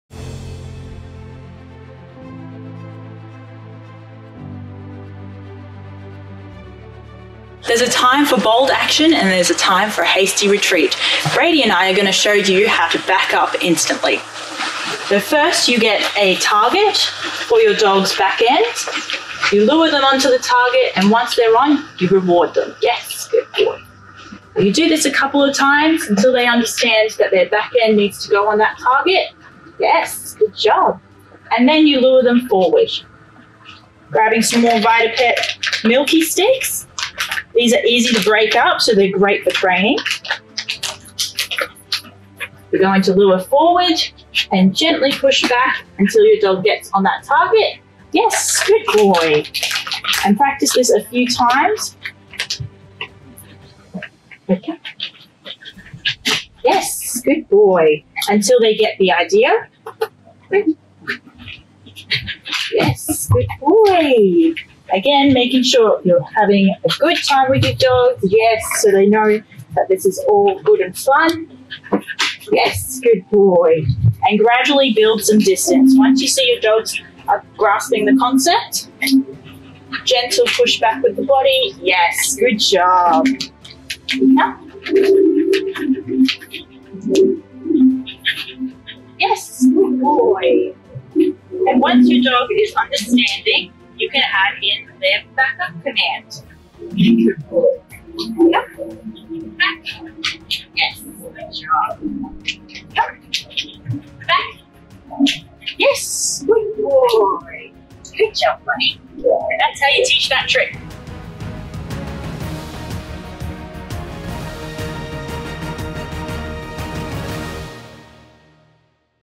How to Teach your Dog to Back Up or Retreat
There's often a need for being able to ask your dog to back up or retreat. Divina shows how to get your dog to back up on command.